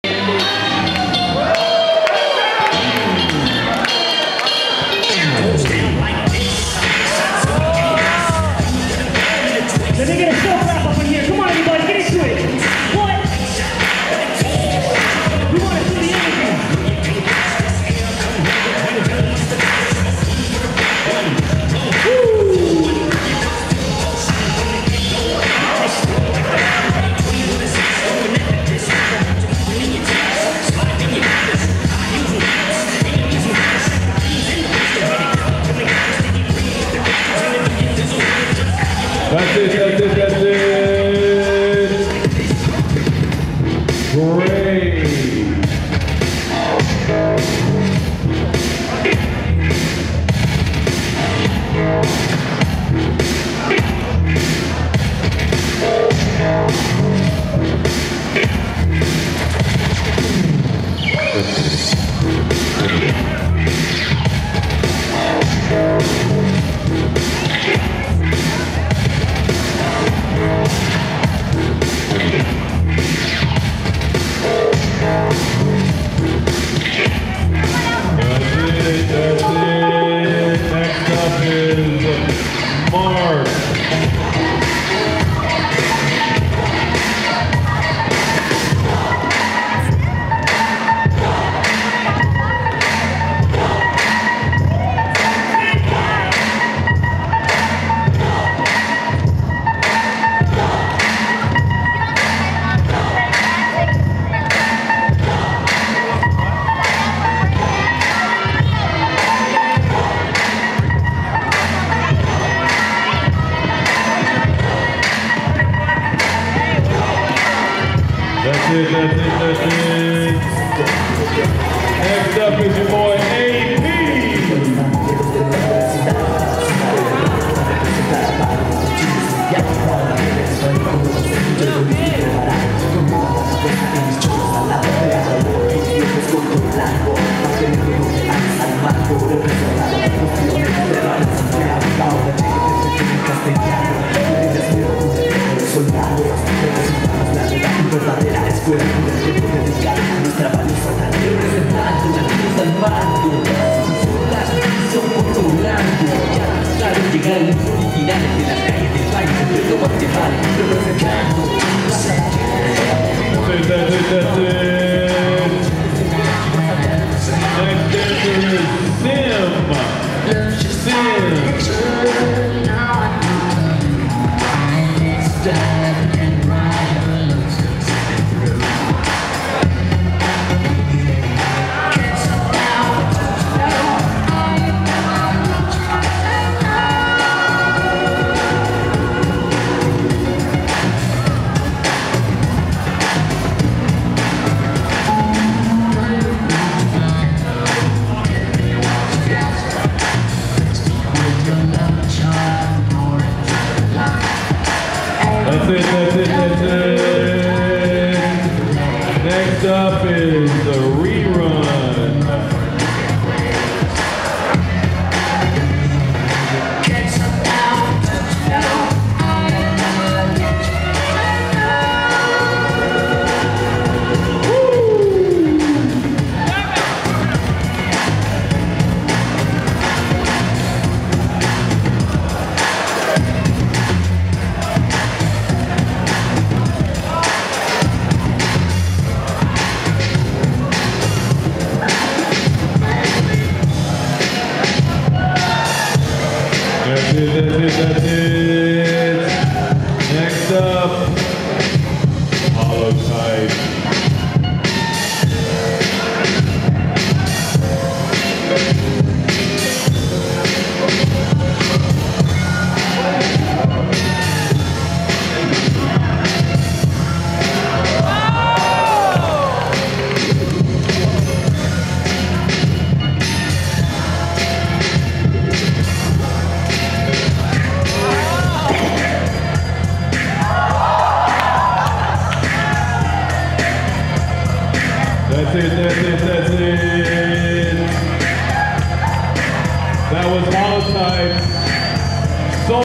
Popping Prelims, Group 4.1
Jedi, Grey, Mark, A.P., Sim, ReRun, Holotype at the YA Summer Jam on Friday, August 3, 2012 at Trinity Church in Houston, Texas with DJ Psycho